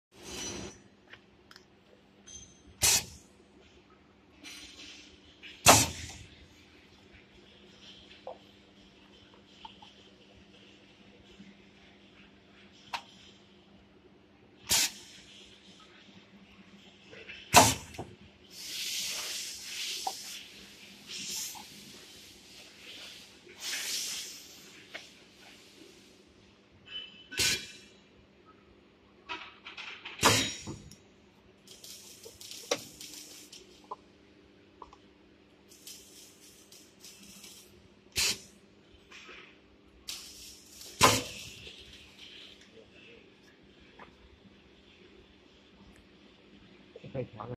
manual handle sealer machine for paper cup with handles
This machine is used for fixing handle manually on paper cup.with a very reasonable and law coast. for normal production machine is very good .